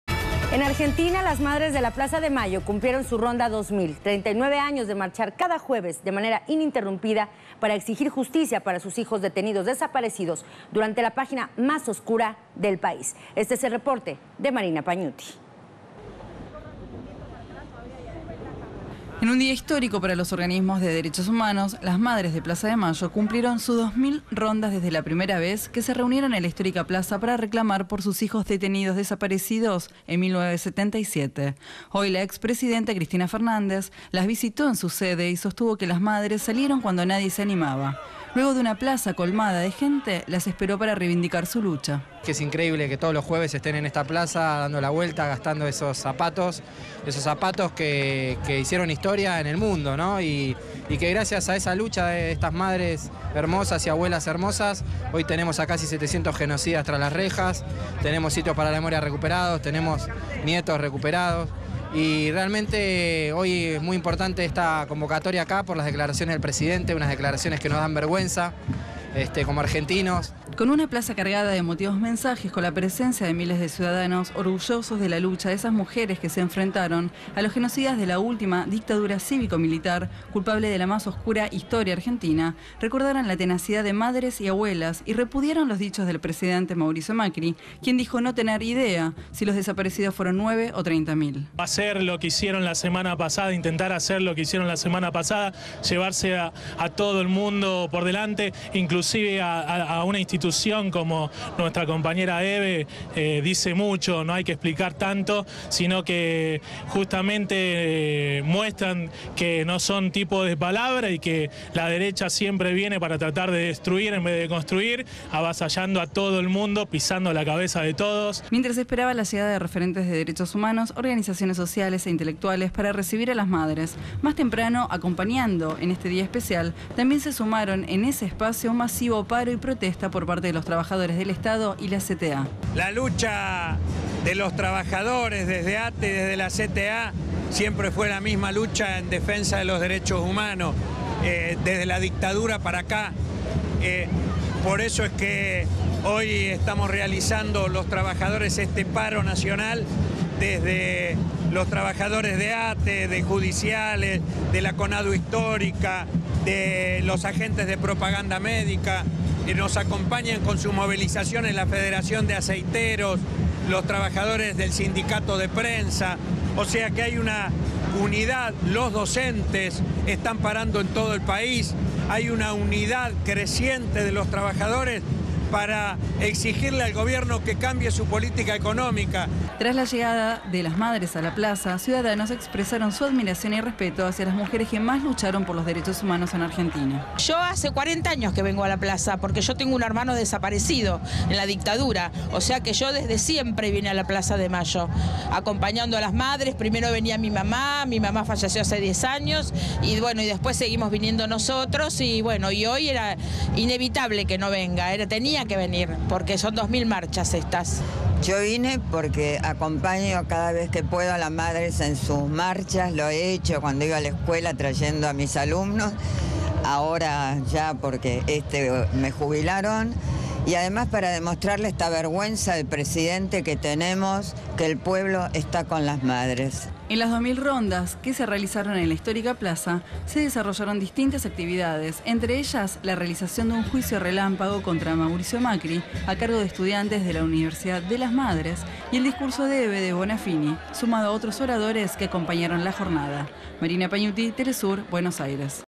Argentina: CFK acompaña a Madres de Plaza de Mayo en ronda 2 mil
VIERNES 12 DE AGOSTO, 2016

Corresponsal: Marina Pagnutti Argentina Dictaduras, memoria y justicia Agenda abierta Selección del editor

Este jueves las Madres de Plaza de Mayo cumplieron dos mil rondas en la plaza de Buenos Aires para exigir la aparición con vida de cientos de personas desaparecidas en el país durante el periodo de dictadura. La ex presidenta del país, Cristina Fernández de Kirchner las visitó en su sede. teleSUR